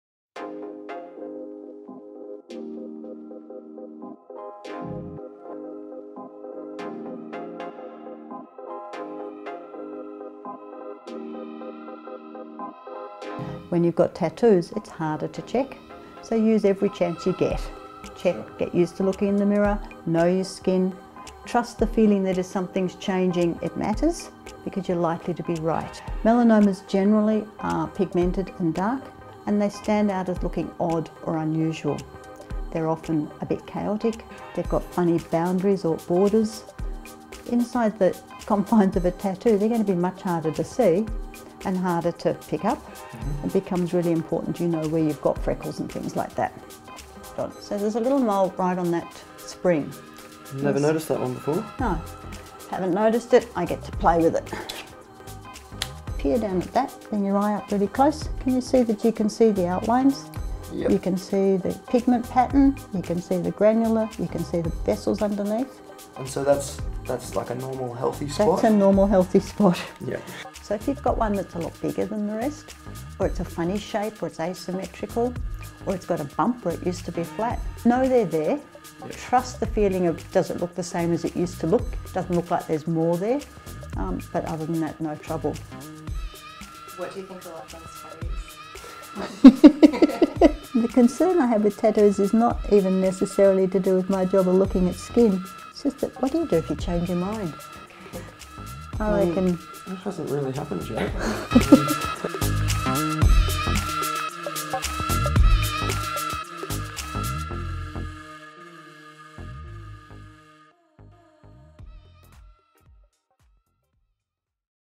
HACK: Tattoos hiding signs of skin cancer
Have you got tattoos? You might wanna look closely at what's underneath 'em - they can hide the signs of skin cancer. We took Lochlan Watt from The Racket for a skin check up.